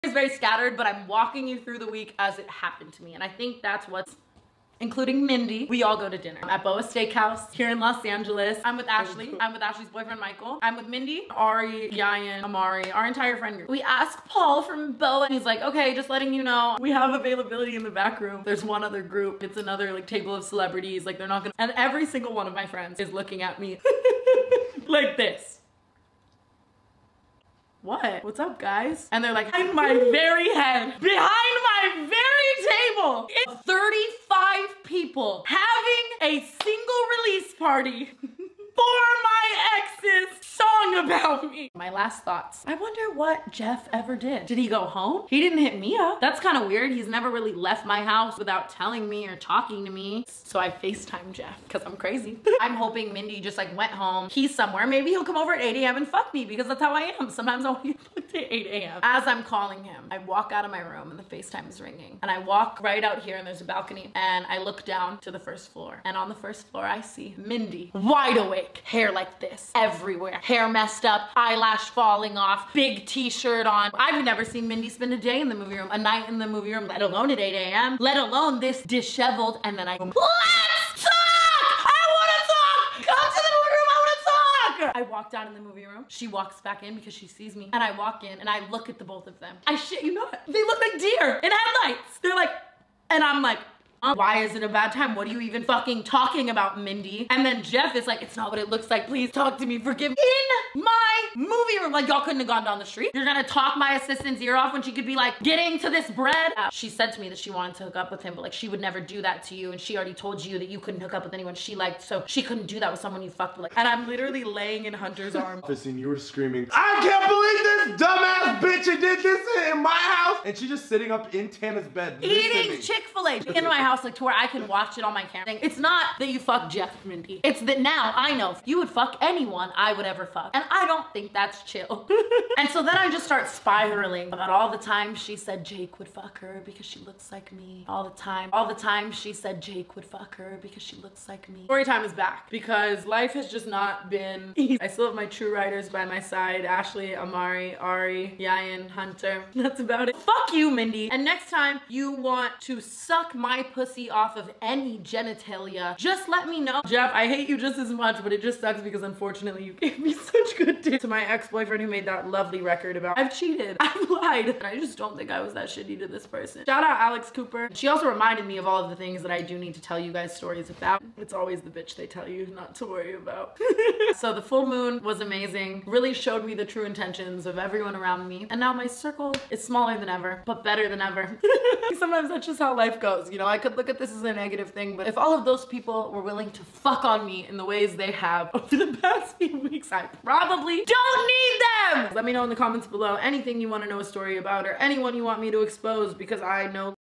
TANA MONGEAU STORYTIME ABOUT BROOKE , MODSUN, JOSH GREKIN?? ( Worst week of my life )
Cut down version on Tabs mongeaus story time about her best friend hooking up with her ex.